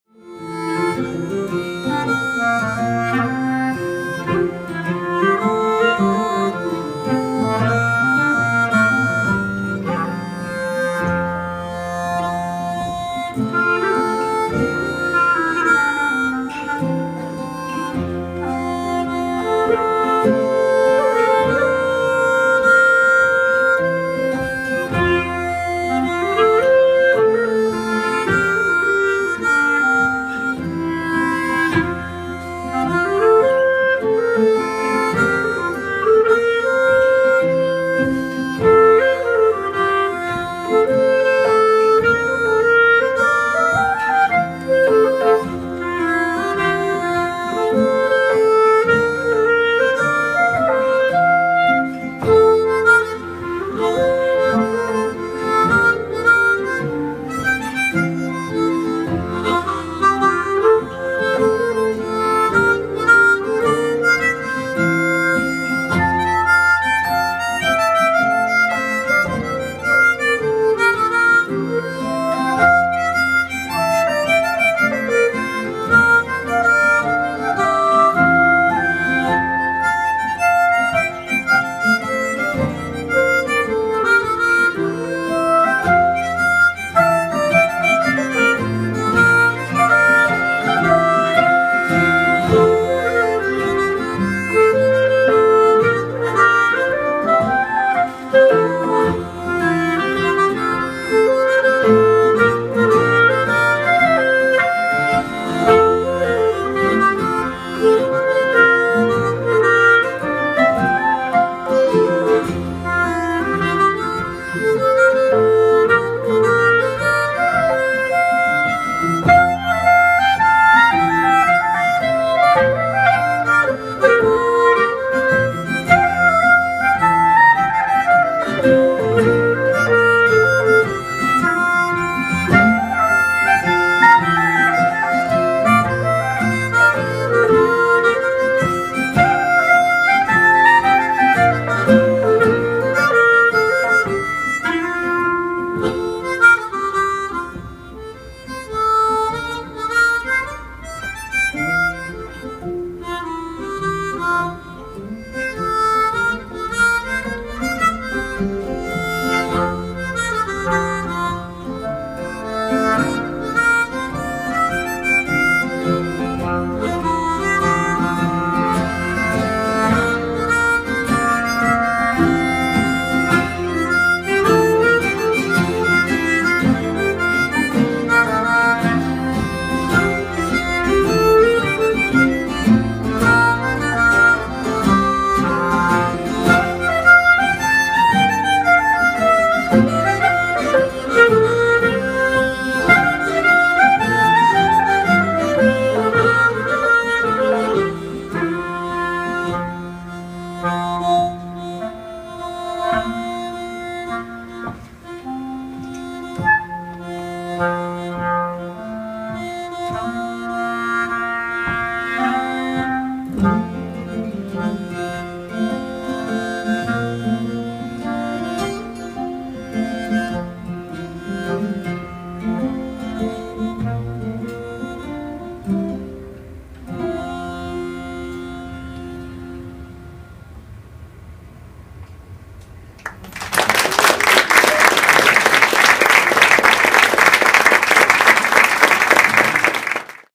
Västanvinden - gränslandsmusik från Värmland och Jämtland.
Erland Westerström – munspel
Albin Myrin – klarinett
Erik Jacobsson – cittern
Tradmusik på udda sättning.
Med den unika sättningen munspel, klarinett och cittern framför Västanvinden traditionella låtar på ett sätt som du inte hört dem förut.
Kopplingen mellan dans och spel är viktigt för alla medlemmarna och har varit ett uttalat fokus sedan starten. I valet av låtar blickar de gärna västerut och har en förkärlek för gränslandsmusiken från Värmland och Jämtland.